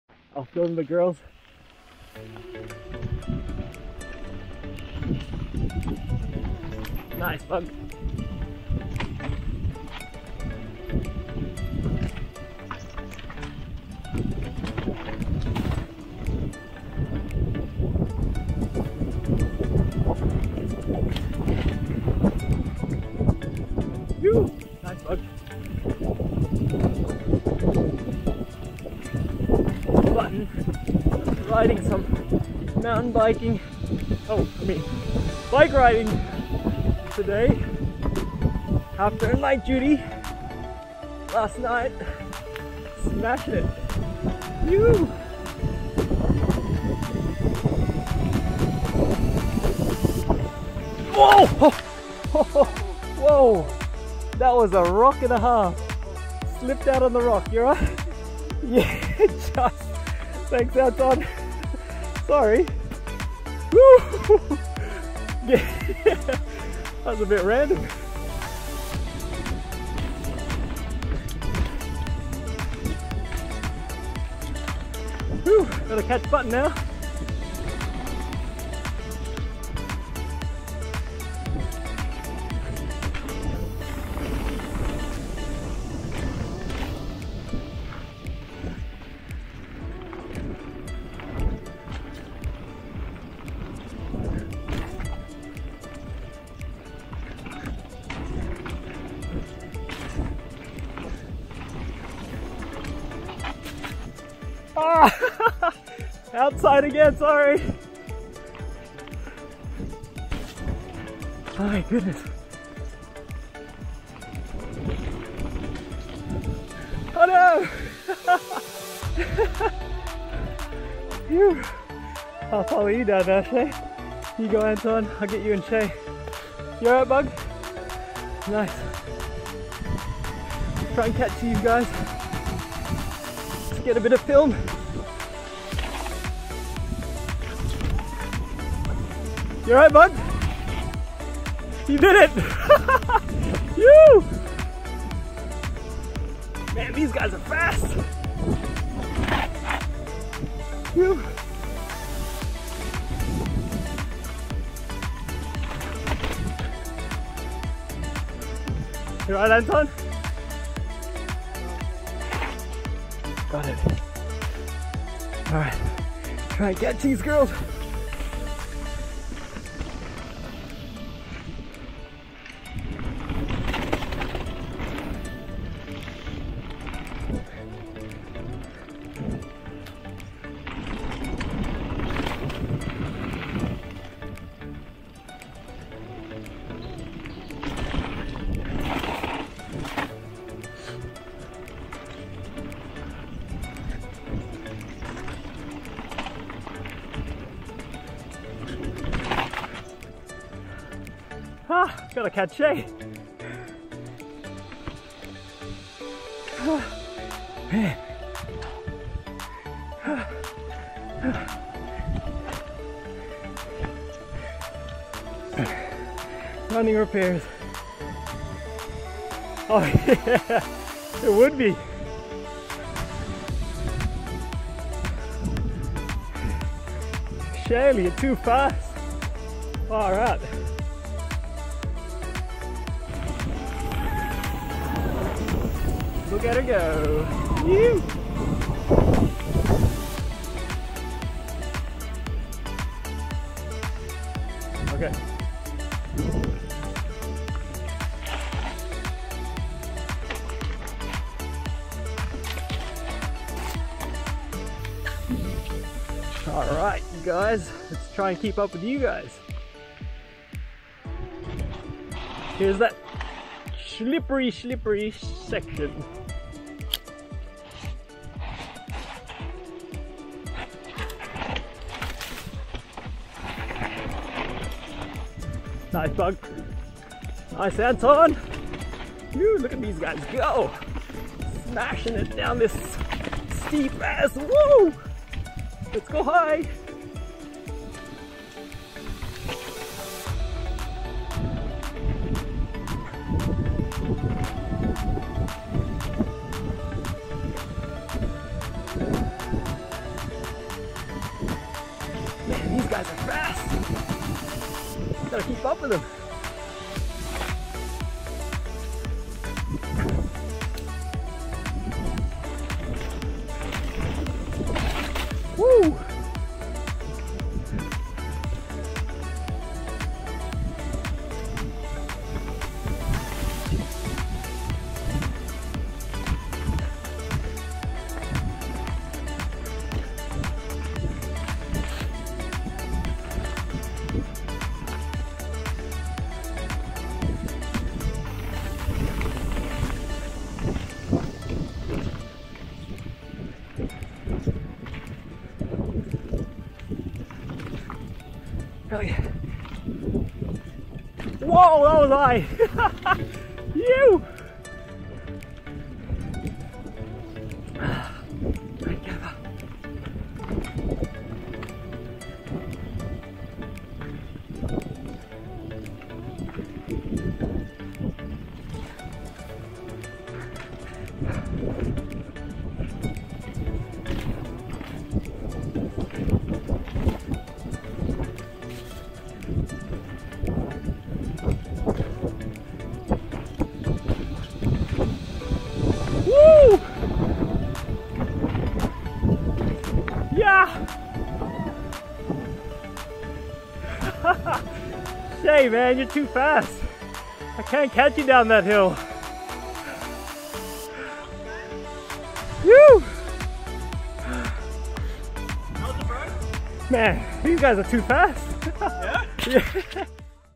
Amaroo Daz Shay Button Anton POV Run @ Ourimbah CC MTB Park
Darryl Takes the Cam and follows people down Amaroo